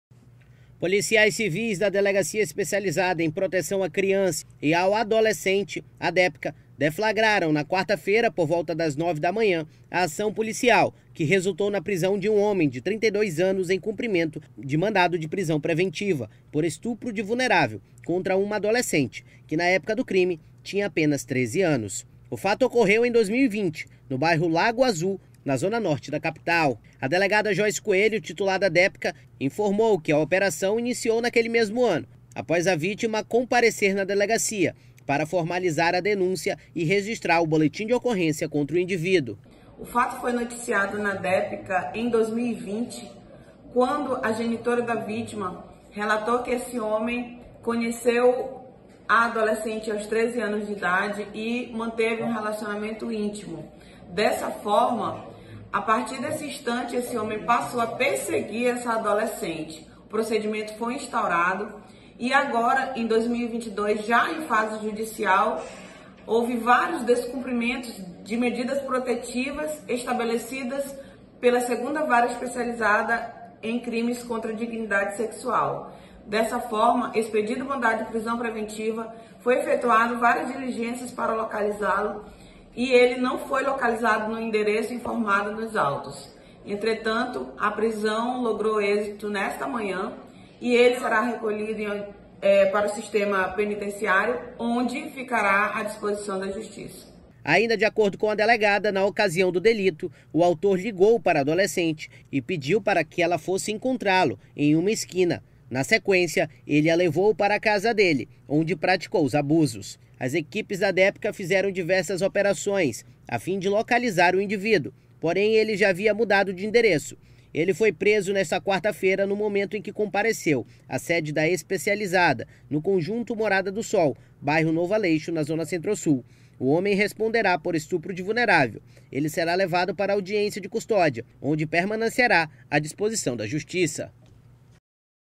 Preso por estupro
Homem é preso suspeito de estuprar adolescente de 13 anos em 2020